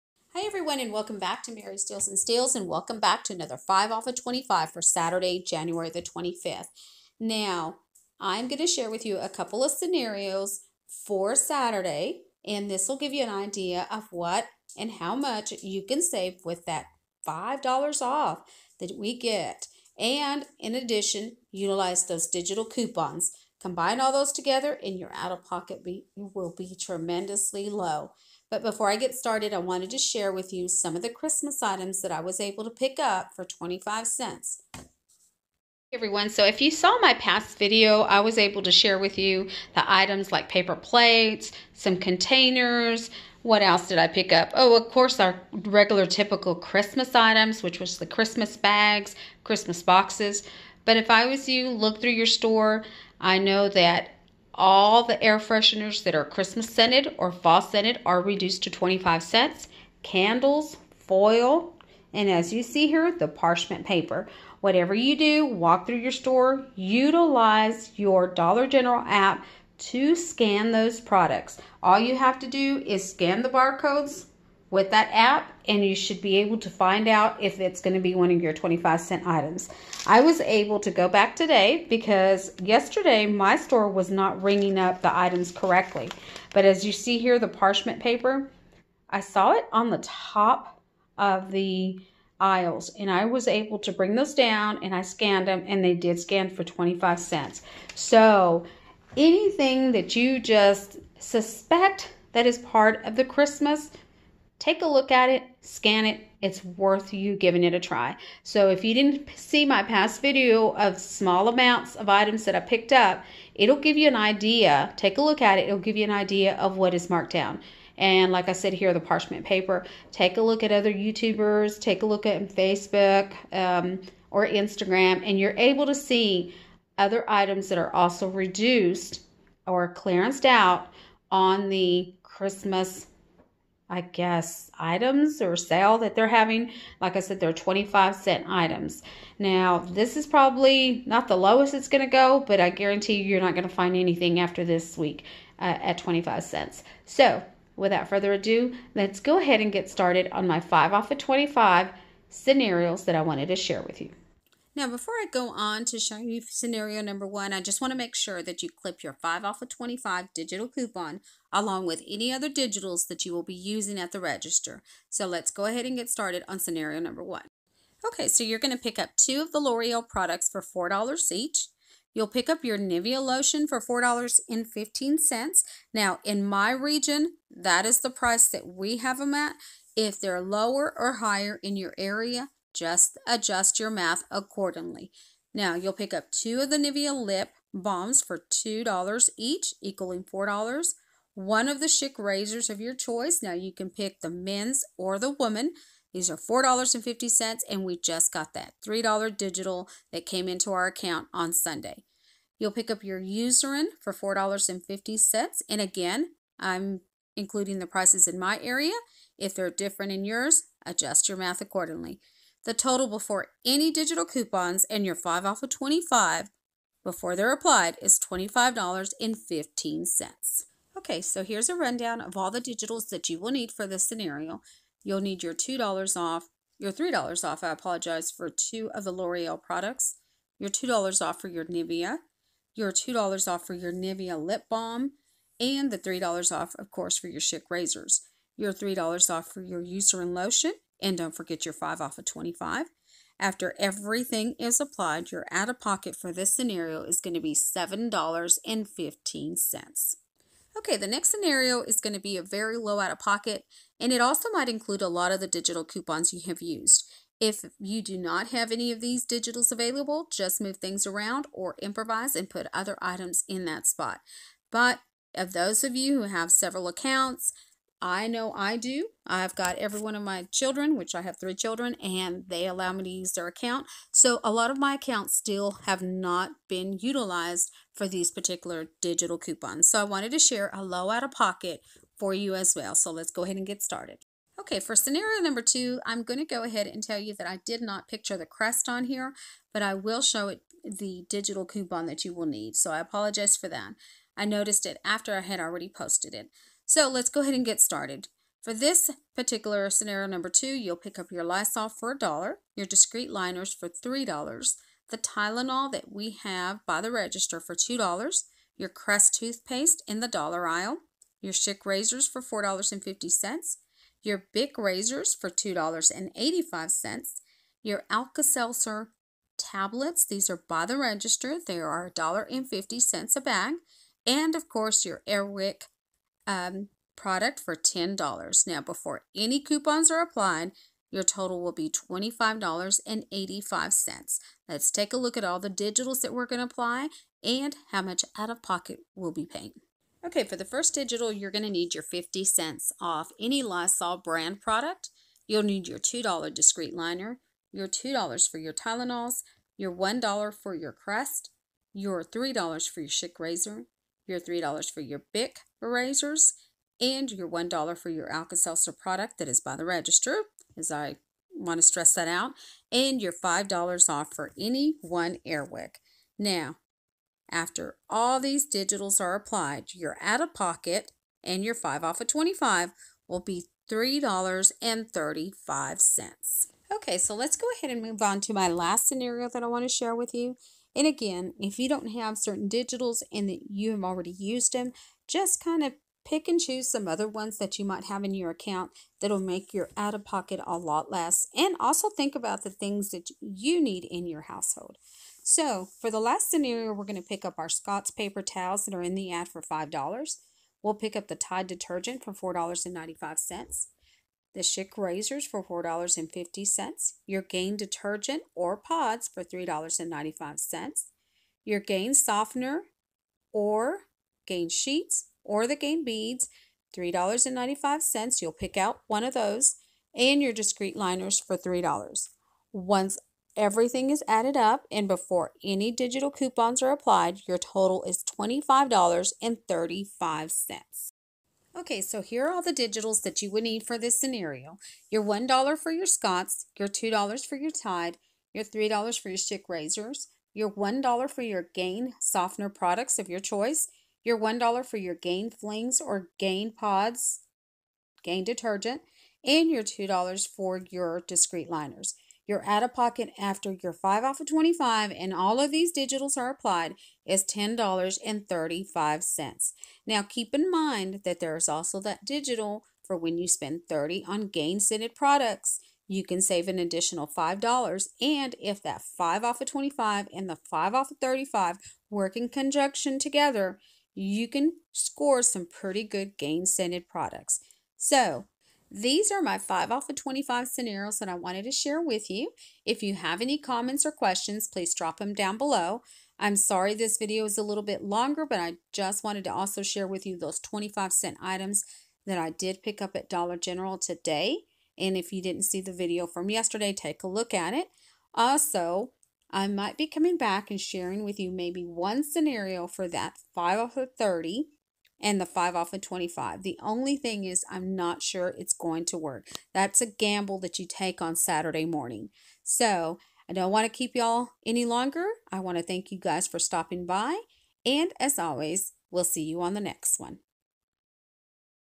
Dollar General 5 off 25 scenarios for 01/25. Low oop as much as 3.35 oop
Dollar General 5/25 scenarios for 01/25. Low oop as low as 3.35

Join Fetch Rewards and earn 2,000 points on your first purchase you upload